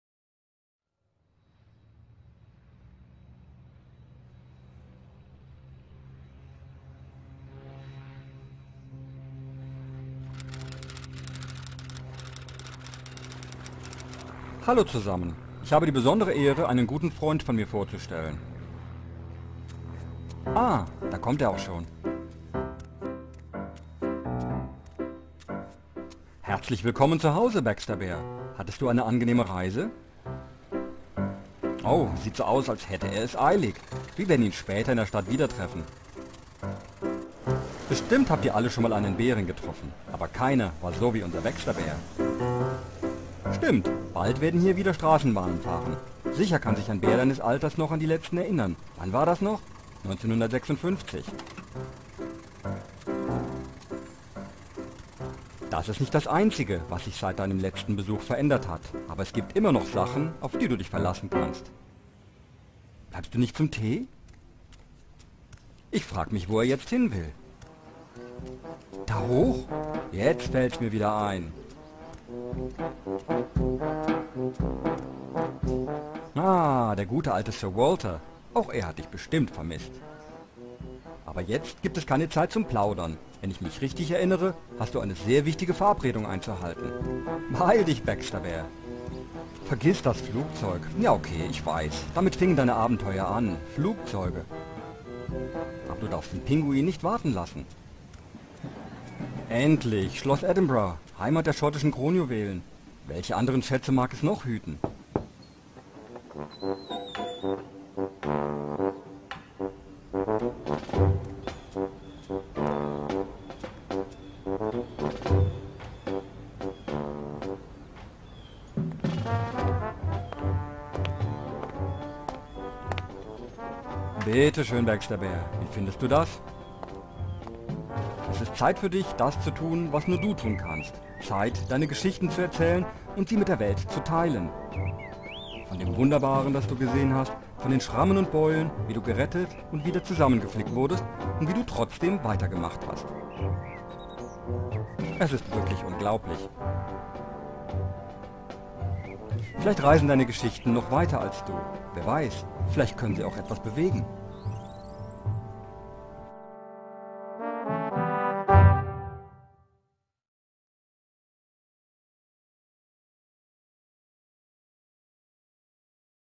Baxterbears Rückkehr
Baxterbear returns to is home town of Edinburgh to write his tales and share them with the world.